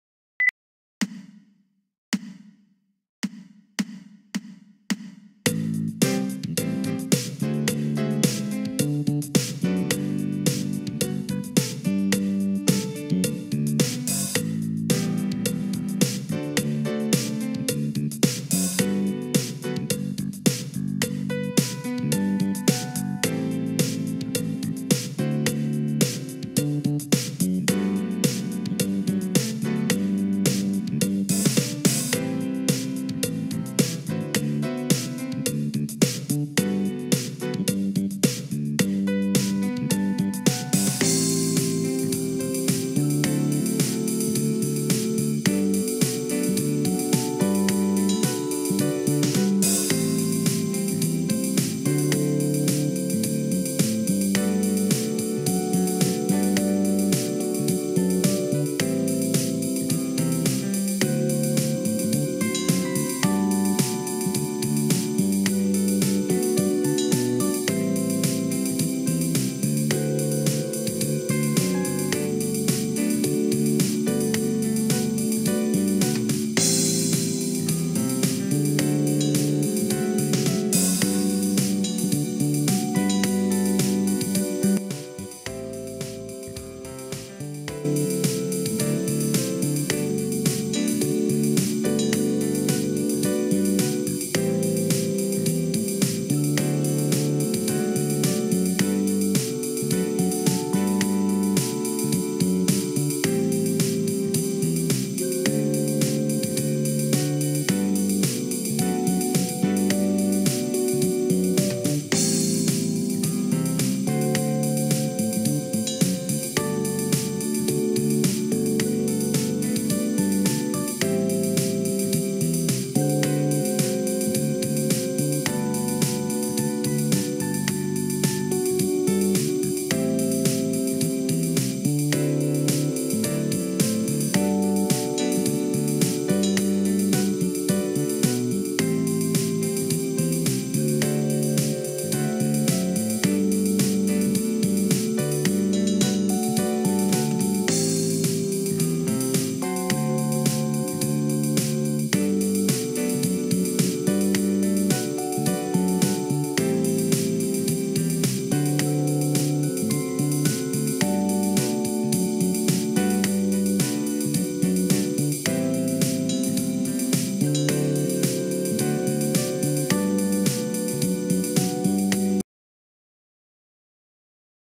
Figure 13 a 2 5 1 Progression in G Major with Faster Moving Chords
This matches Figure 13 in my book Patterns A La Modes.  Practice playing the A Dorian over Am7, D Mixolydian over D7, and G Ionian over the Gmaj7. Then try G Dorian over the whole pattern.